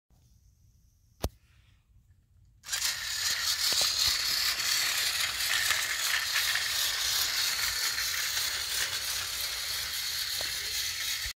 Something I got from Ebay Episode 4
Went ahead and got a TOMY Toad break van. I had to clean him and I wasn't in the mood to show me opening him up due to feeling a little irritated. Not by school this time, but just from outside influences. I feel a bit better and decided to show him off anyway because he'll be important for hopefully my next package.